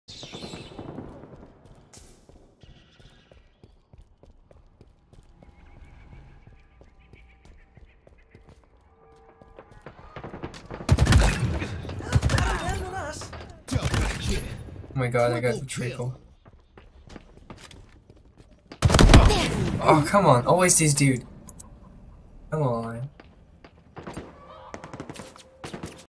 Dude snaps to target like aimbot
Got the triple yeah, but this one dude keeps killing me.